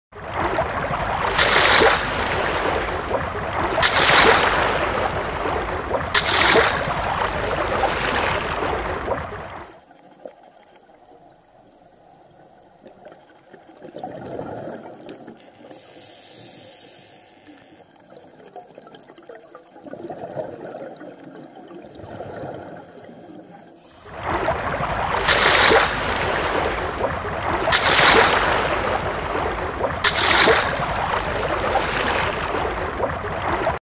Adlerrochen Flugshow
Dieser Adlerrochen zeigt einen wahren Raketenstart. Senkrecht zieht er von seinem Startplatz  in das Blau des Roten Meeres. Jede Flugshow würde wahrscheinlich neidisch werden bei solch eleganten Manövern.